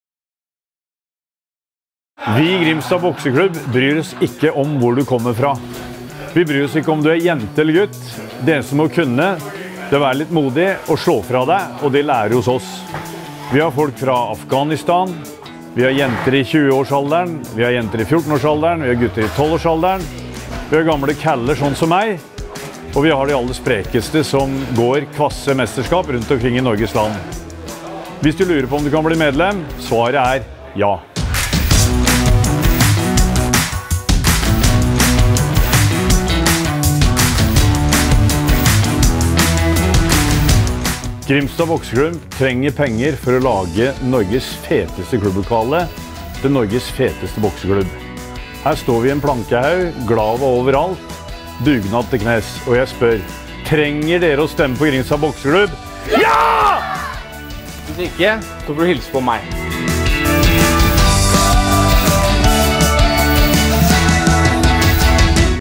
Grimstad Bokseklubb - Drømmefinalist 2018
Grimstad Bokseklubb – drømmer om å få til Norges tøffeste klubblokale for boksing. Grimstad Bokseklubb er et samlingspunkt for alle barn, ungdom og voksene som er interesserte i denne idretten. Klubben har flinke trenere som bidrar til at alle føler seg velkommen i det gode miljøet klubben har. Nå jobbes det på spreng for å få på plass nye treningslokaler.